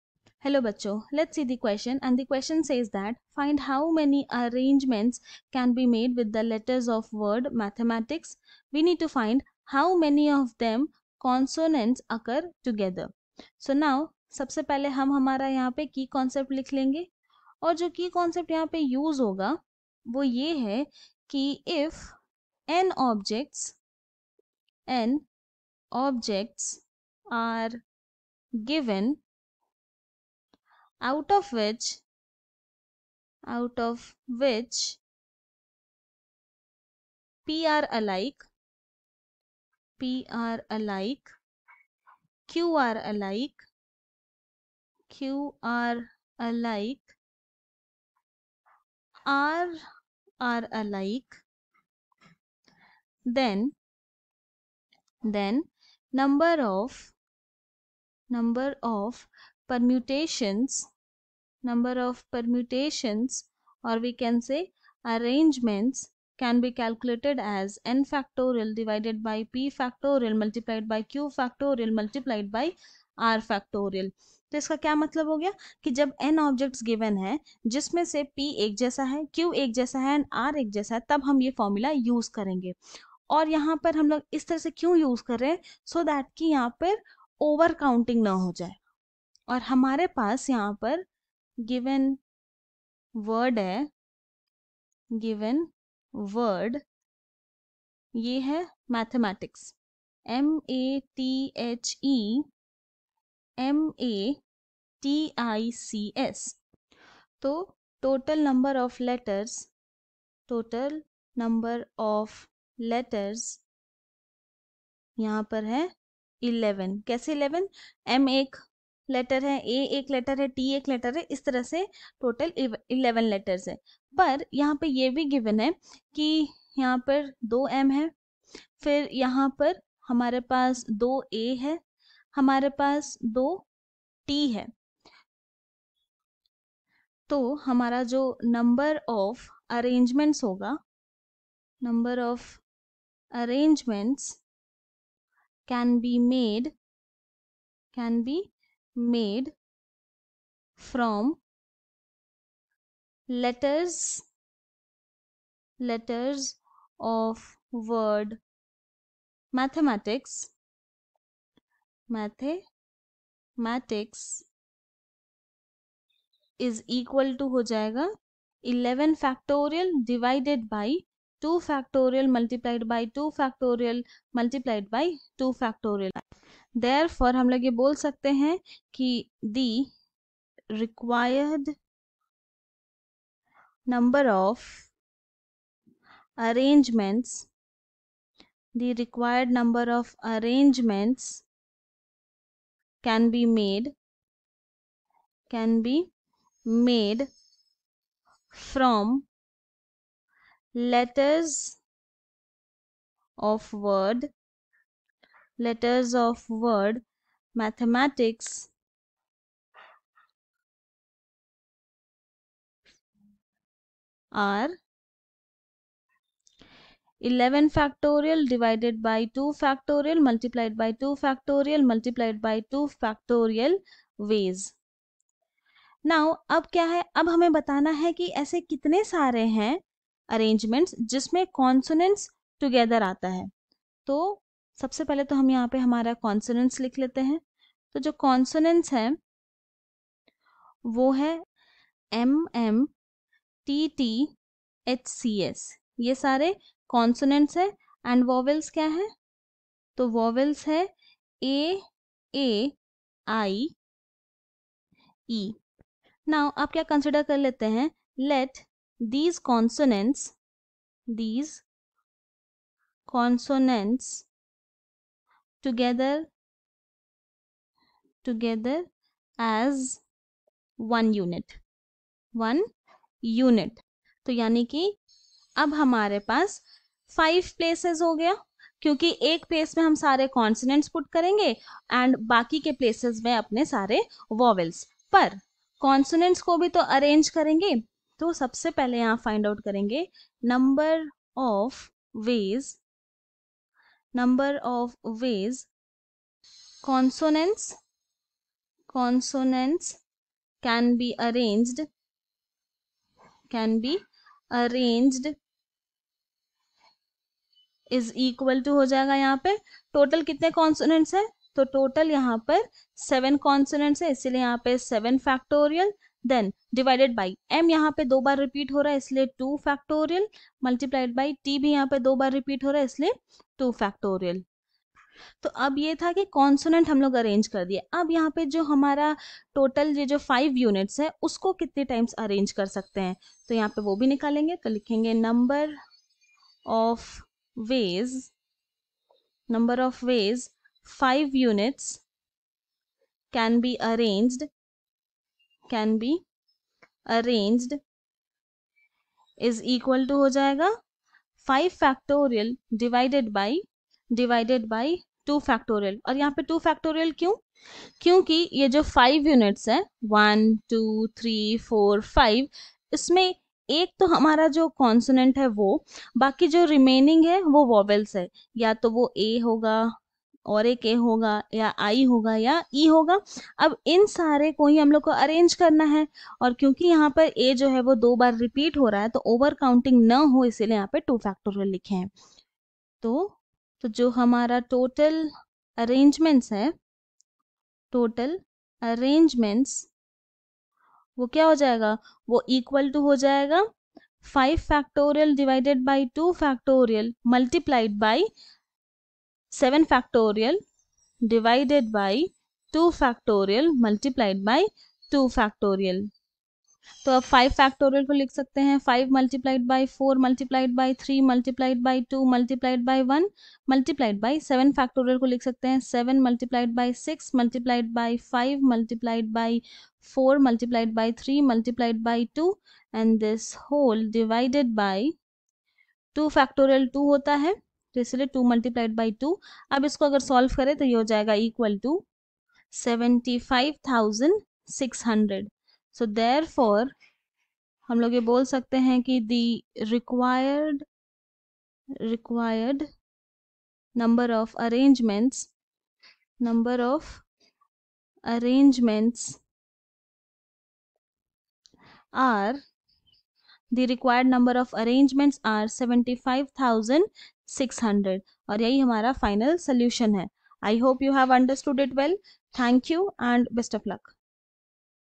Find how many arrangements can be made with the letters of the word 'MATHEMATICS' ? In how many o...
Find how many arrangements can be made with the letters of the word 'MATHEMATICS' ? In how many of them:
(i) consonants occur together,